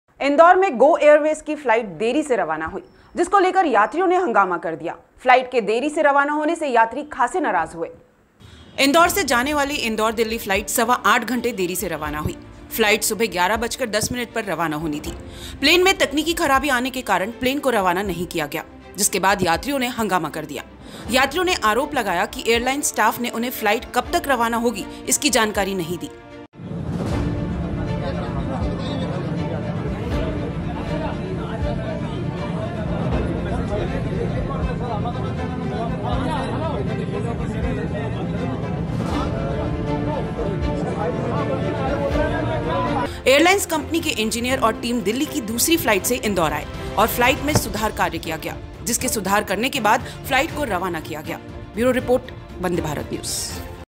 गो-एयरवेज की फ्लाइट देरी से रवाना हुई, यात्री हुए परेशान, किया हंगामा। Vande Bharat News
इंदौर में गो-एयरवेज की फ्लाइट देरी से रवाना हुई, जिसको लेकर यात्रियों ने हंगामा कर दिया। फ्लाइट के देरी से रवाना होने से यात्री खासे नाराज हुए।